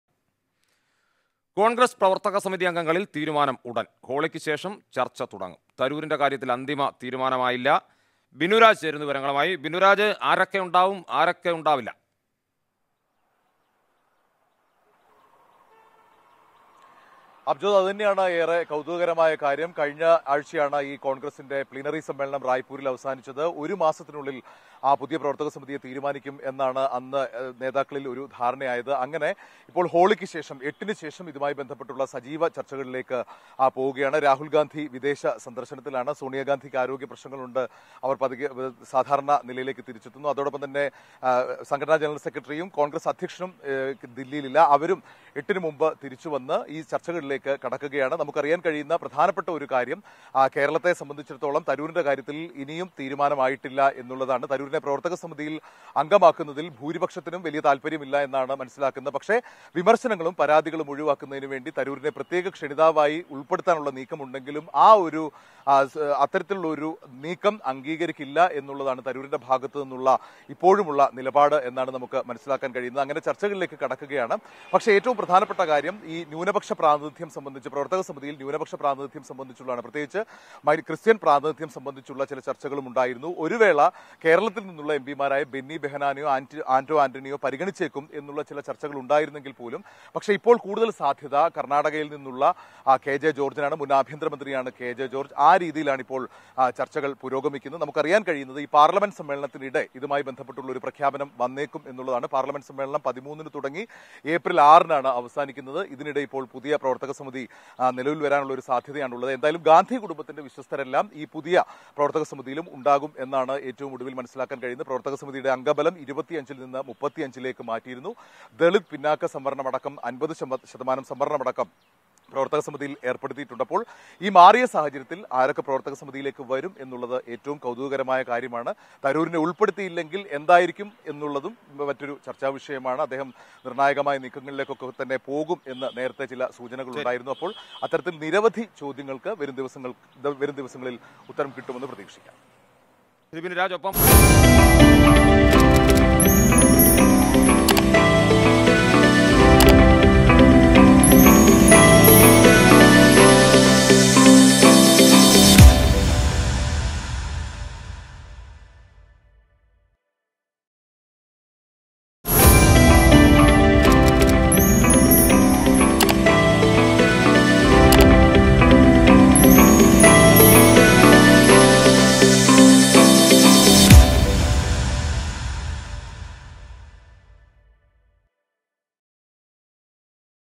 കോൺ​ഗ്രസ് പ്രവർത്തക സമിതി അം​ഗങ്ങളിൽ ഉടൻ തീരുമാനം, ഹോളിക്ക് ശേഷം ചർച്ച തുടങ്ങും
കോൺ​ഗ്രസ് പ്രവർത്തക സമിതി അം​ഗങ്ങളിൽ ഉടൻ തീരുമാനം, ഹോളിക്ക് ശേഷം ചർച്ച തുടങ്ങും; തരൂരിന്റെ കാര്യത്തിൽ അന്തിമ തീരുമാനമായില്ല
Congress working committie | Sonia Gandhi | Rahul Gandhi